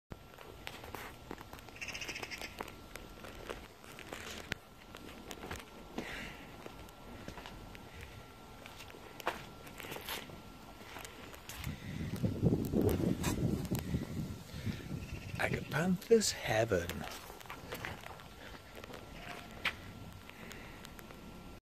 Agapanthus Heaven
We love Agapanthus here at Dreamscape, so when we're asked to build them into a planting plan, or when our designers specify them for a client, we are very happy Dreamscapers